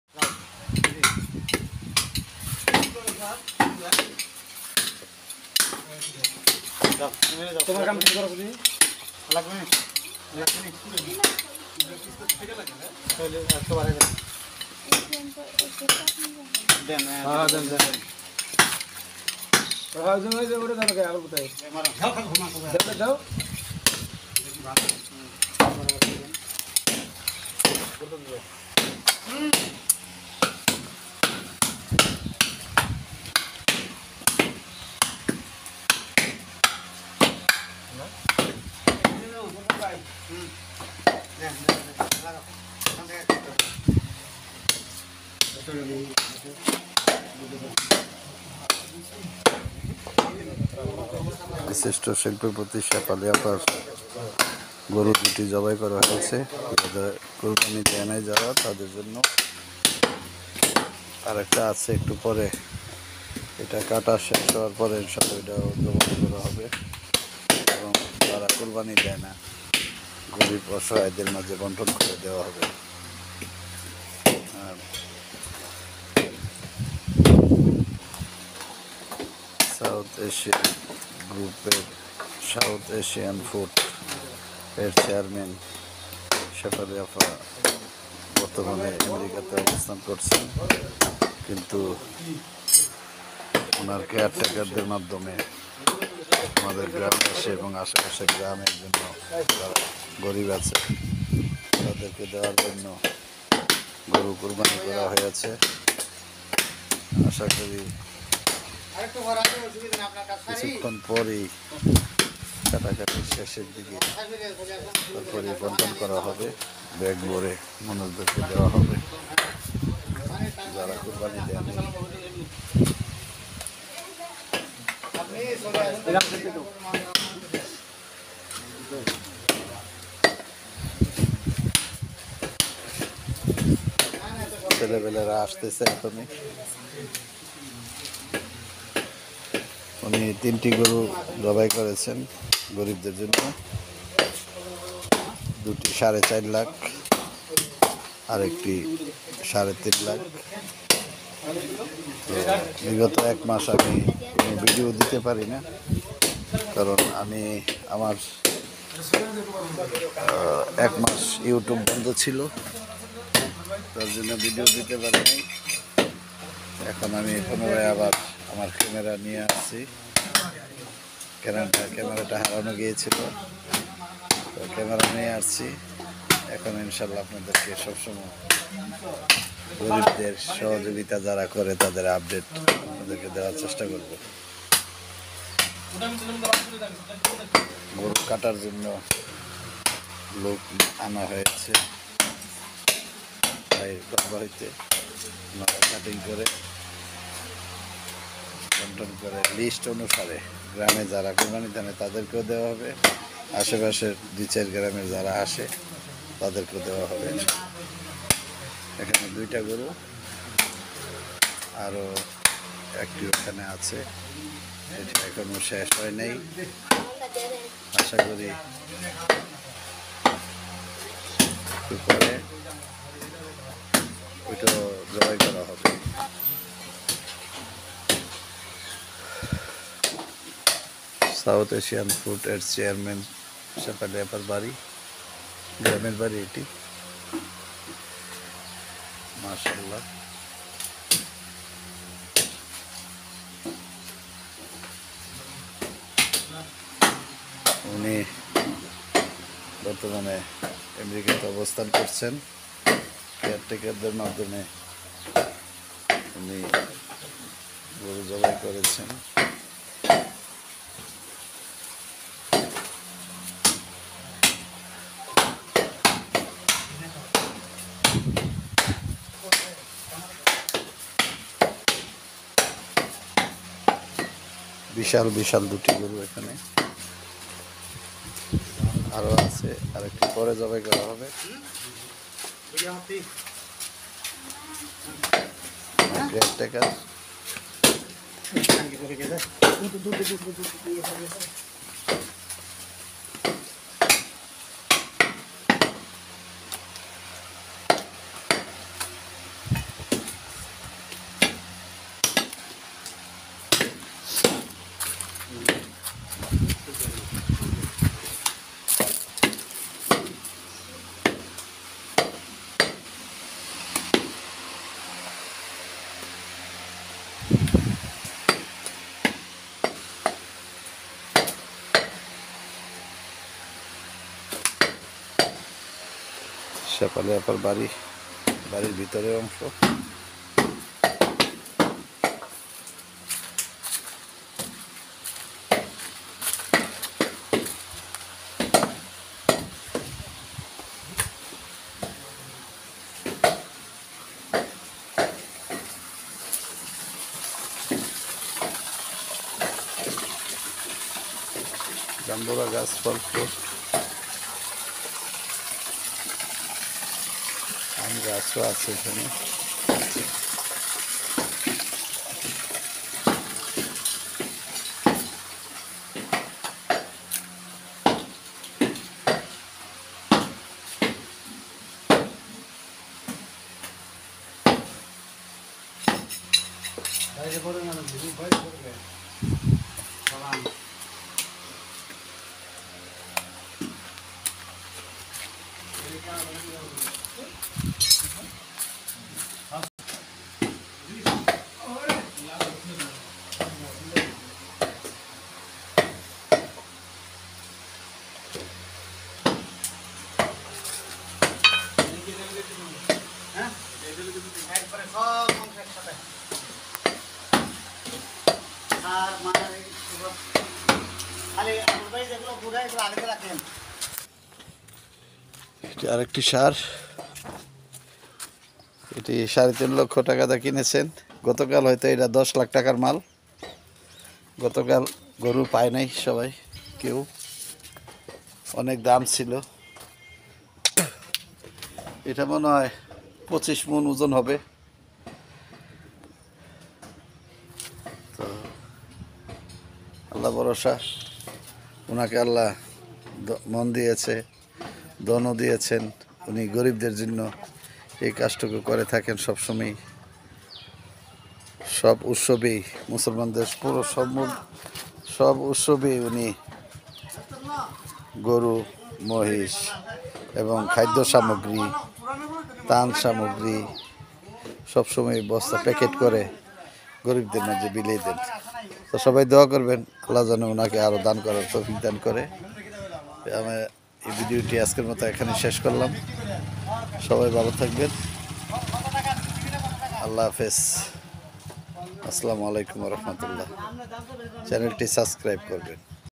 বিশাল আকারে তিনটি কুরবানীর পশু গরীবদের মাঝে দান করেছেন সাউথ এশিয়ান ফোর্সের চেয়ারম্যান শেফালী আপা
তালতলা গ্রামবাসী সাউথ এশিয়ান ফোর্সের চেয়ারম্যান বিশিষ্ট শিল্পপতি দানবীর খাদিজা বেগম শেফালী আপা সবসময়ই গরীব অসহায়দের মাঝে কুরবানীর গোশত বন্টন করেন। আমাদের তালতলা গ্রামের একমাত্র দানবীর বিশিষ্ট শিল্পপতি শেফালী আপা প্রতিবছরই উনি বিদেশে থাকা কালিন ওনার কেয়ার টেকারদের সহযোগিতা তালতলার গরীব অসহায় জনসাধারণের মাঝে ঘরে ঘরে কোরবানির গোশত পৌঁছাইয়া দেওয়া হয় আল্লাহ তাআলা উনার দানকে কবুল করুন আমীন।